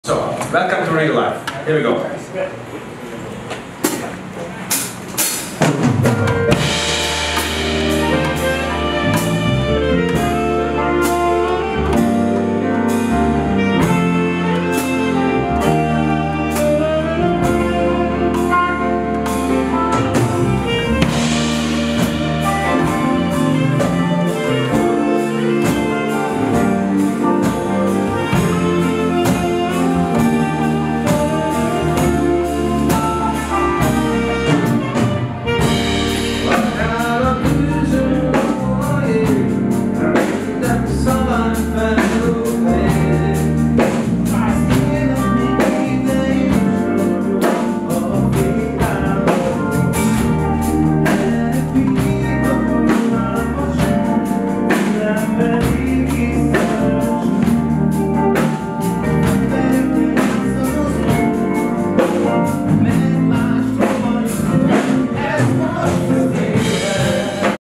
Mandoki Soulmates főpróbán , MANDOKI és KARAMELL részlet.
Esemény: Mandoki Soulmates Főpróba
Dátum: 2023. augusztus 17-én, 17:00 órai kezdettel
Helyszín: 1011. Budapest, Bem rakpart 6.

A Mandoki Soulmates augusztus 18-i koncertjéhez tartozó főpróba kiemelkedő vendégművészek részvételével zajlik, köztük olyan neves magyar előadók lesznek jelen mint Caramel, Mező Misi, és nem utolsó sorban Zséda.

A világhírű zenészek között láthatjuk Leslie Mandokit, Al Di Meolát, Richard Bonát, Till Brönner-t, Nick van Eedét, Bill Evanst, Mike Sternt, Randy Breckert, John Helliwellt, Tony Carey-t. Idén lép színpadra a legnagyobb amerikai- angol soulmates csapat.

Videó : Göbölyös László